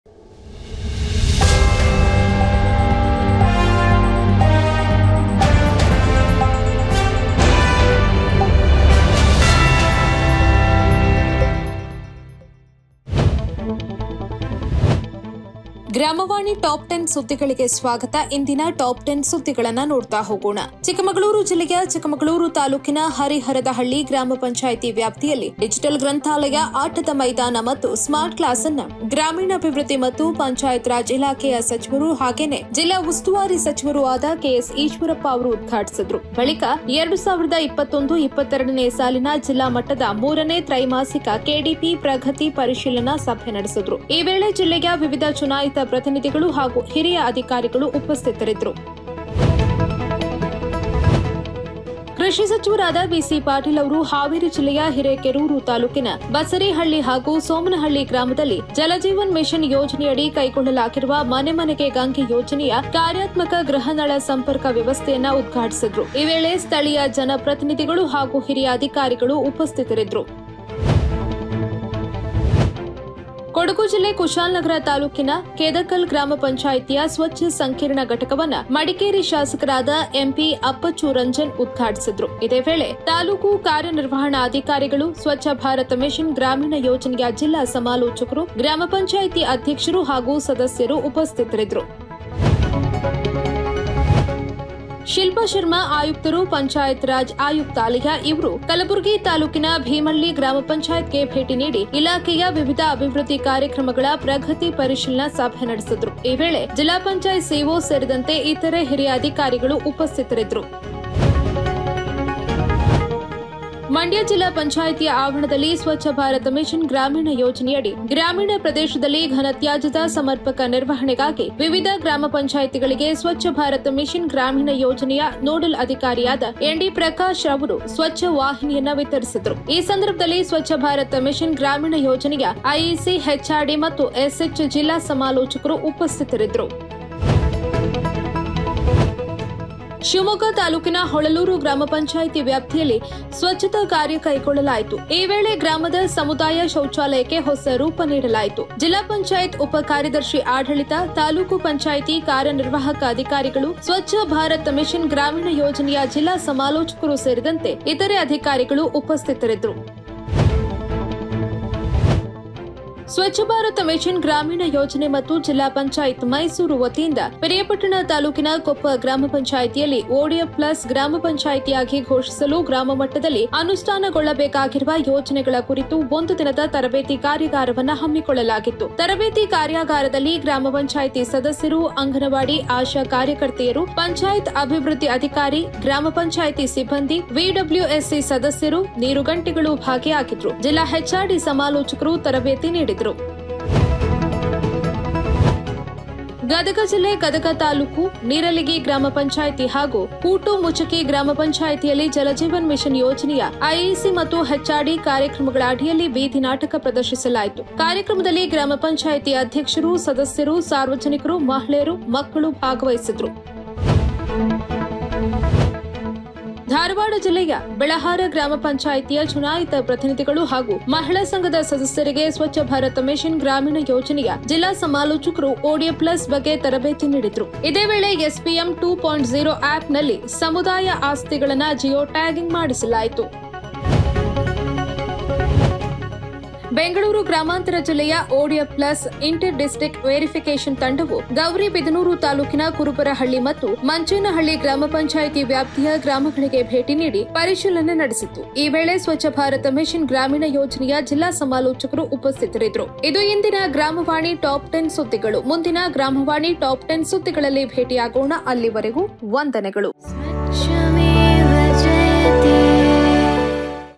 Episode 27-ಗ್ರಾಮವಾಣಿ ಟಾಪ್-10 ಸುದ್ದಿ@PublicTV@aajtak@DoordarshanChandana@DDnews​
ಗ್ರಾಮೀಣ ಕುಡಿಯುವ ನೀರು ಮತ್ತು ನೈರ್ಮಲ್ಯ ಇಲಾಖೆಯ ವಿವಿಧ ಕಾರ್ಯಕ್ರಮಗಳ ಕುರಿತಾಗಿ ಮಾಹಿತಿ ನೀಡುವ ಟಾಪ್ 10 ಸುದ್ದಿಗಳು ಇಲ್ಲಿವೆ.